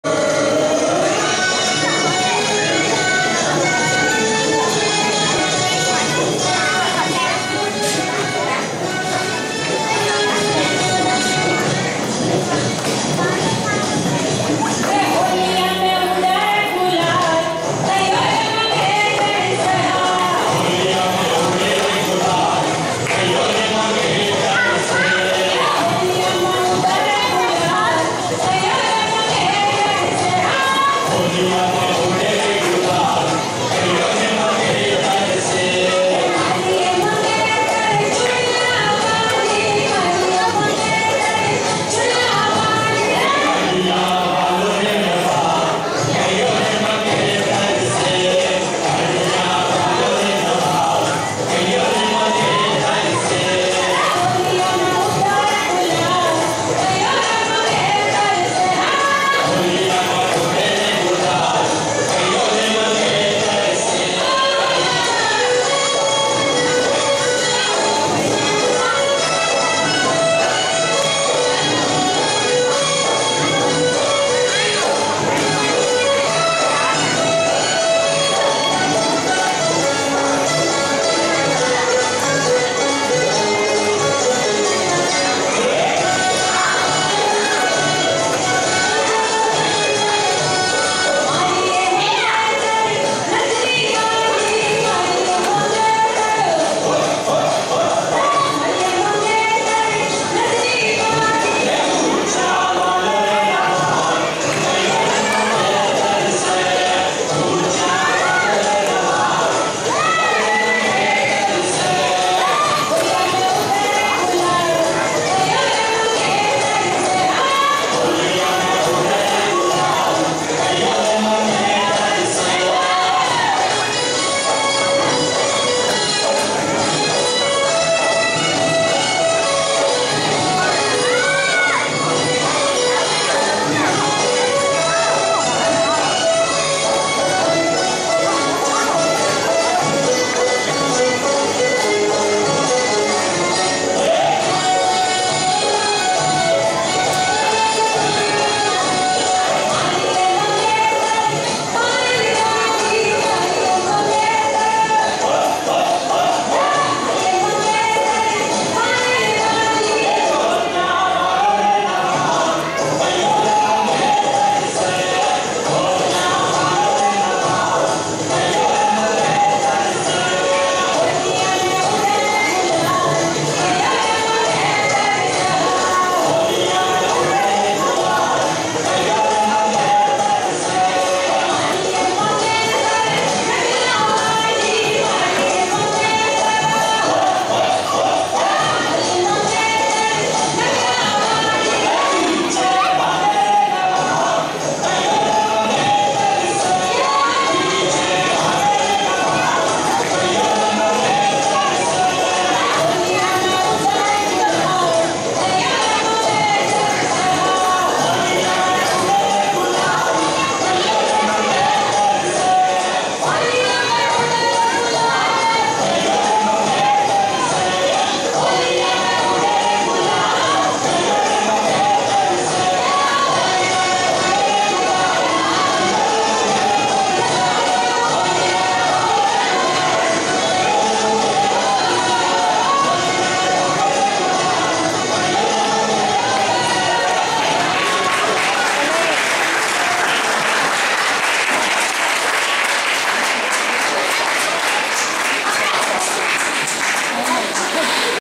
Folk Dance by Rangeela Rockers - Cultural Arts Fair '13, Arlington Heights School District 25
Folk Dance by Rangeela Rockers at the 2013 Cultural Arts Fair of Arlington Heights School District 25.

Performance by: Rajkumar, Aniketh, Kritin, Devika, Nisha and Emlyn

Choreographed by: Muktha